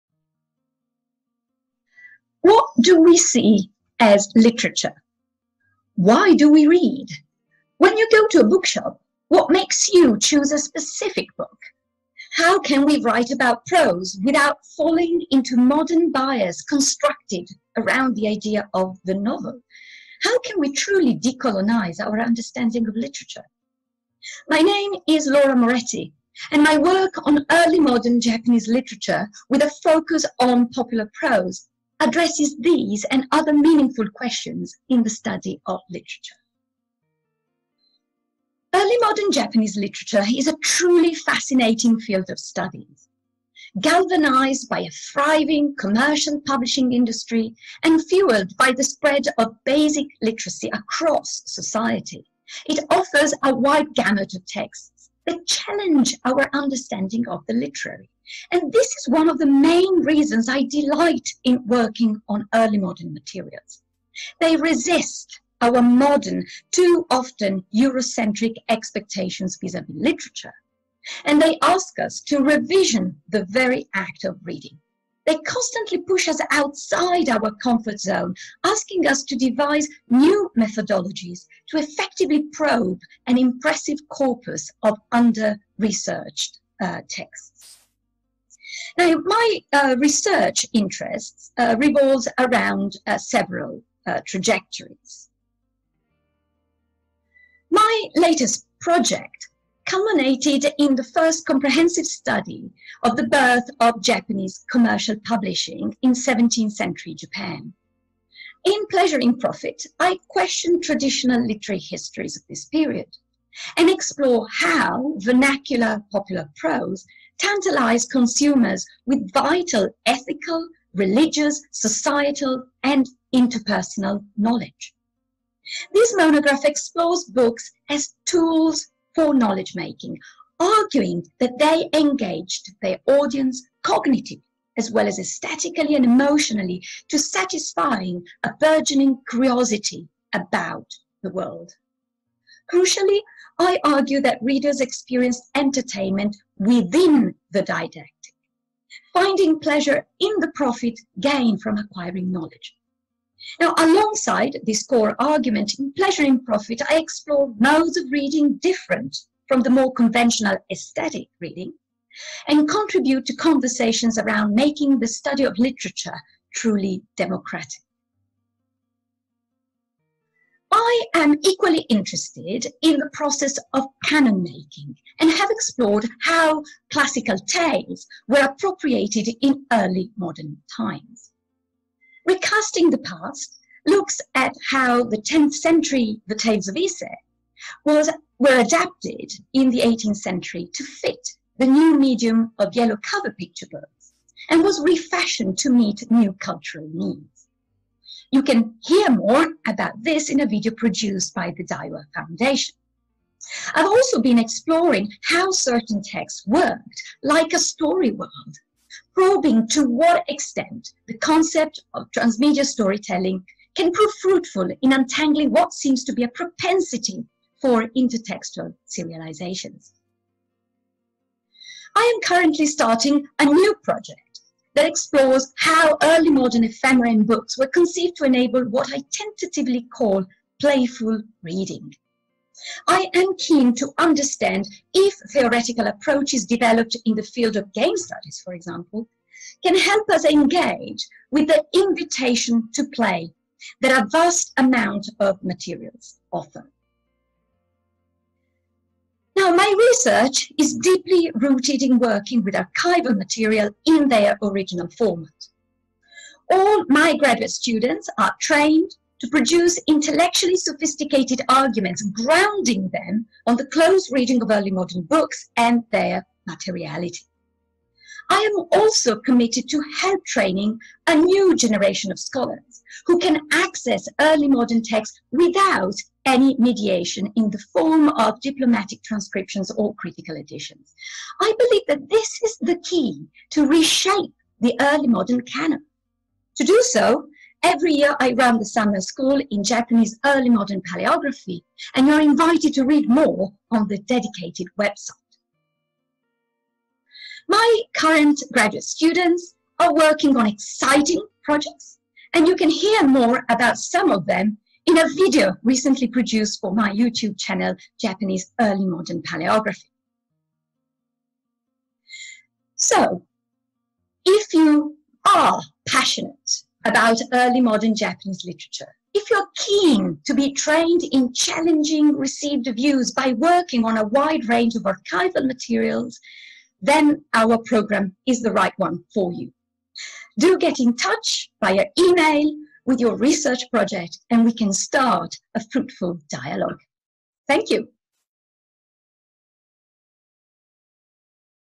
Postgraduate Open Day 2020 - Dr Laura Moretti on Early Modern Japanese Literature
FAMES Postgraduate Open Day 2020:

In this video Dr Laura Moretti talks about her research in the field of Early Modern Japanese Literature, her recent publications and her current research.